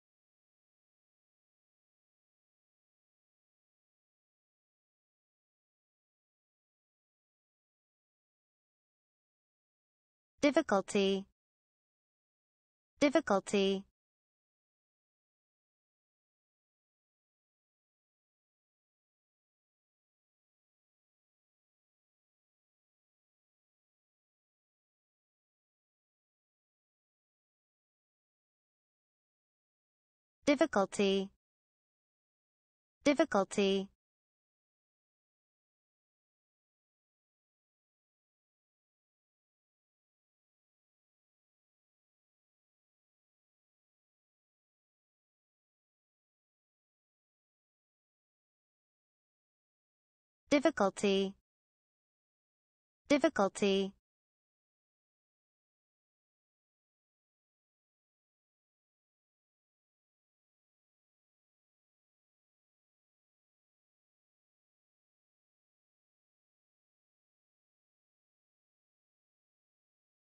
How to say Difficulty in English? - Pronunciation Owl
How to say Difficulty in English? Learn the pronunciation of Difficulty!